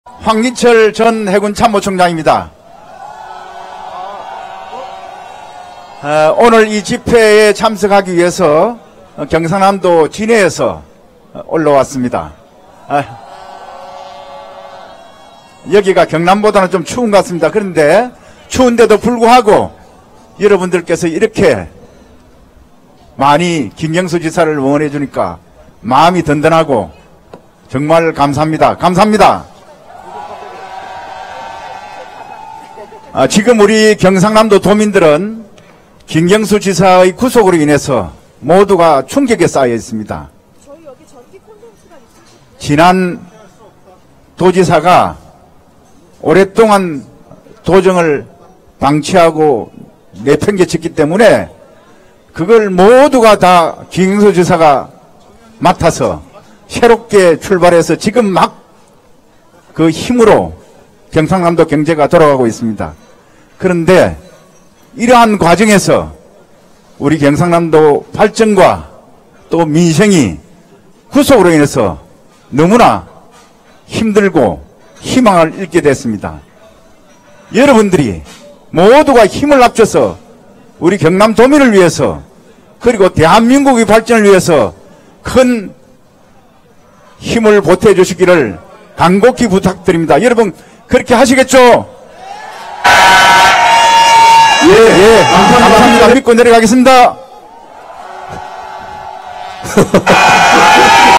경남 진해에서 광화문 촛불집회를 위해 올라온 황기철 前 해군참모총장 [현재 경남은...]
시사타파TV 여러분의 소중한 후원으로 현장과 스튜디오에서 제작됩니다.